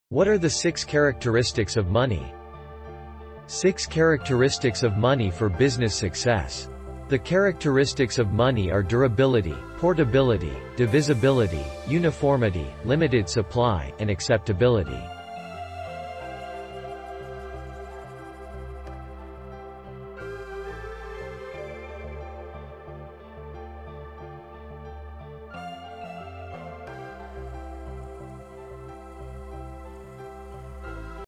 What are the 6 characteristics of money?
The characteristics of money are durability, portability, divisibility, uniformity, limited supply, and acceptability.